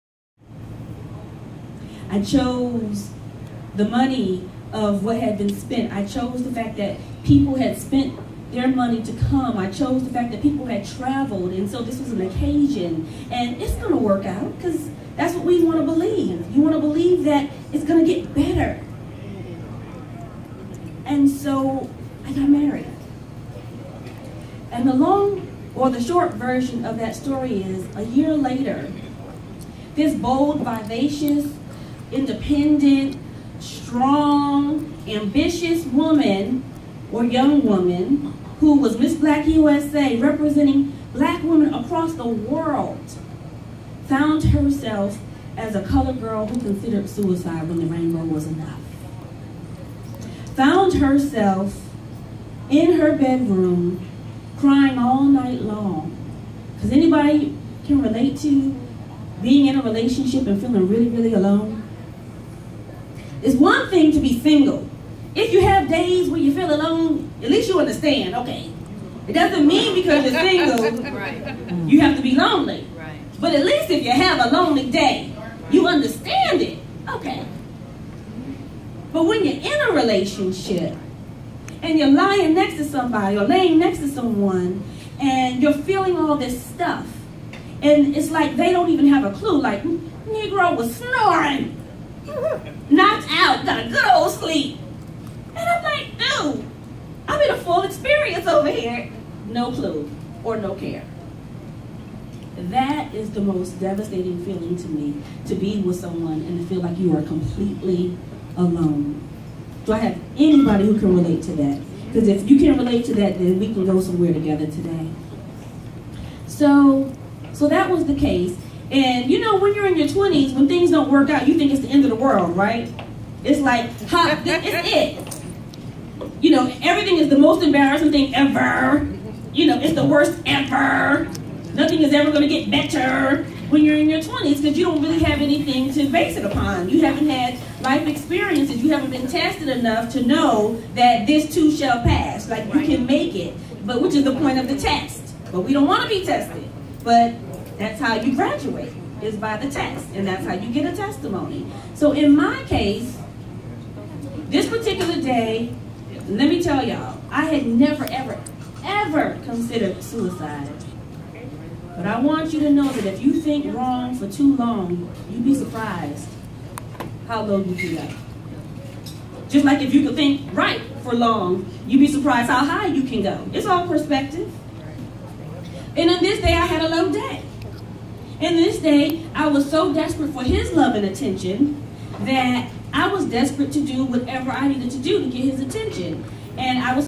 Marching into Your Love Power - Part 3
I must give a shout out to all of the ladies who showed up for our first Man Whisperer event of 2017 in Desoto, Texas! Some of the hot topics that we covered were:

• How being hard vs. soft plays out in our lives;
• What real communication looks like and what it means to become bi-tonal;
• Soft Power: The art of getting want you want and making others feel good about it;
•  Fun Dating Tips and;
• The Secrets to becoming a Man Whisperer

for details on the next Man Whisperer event.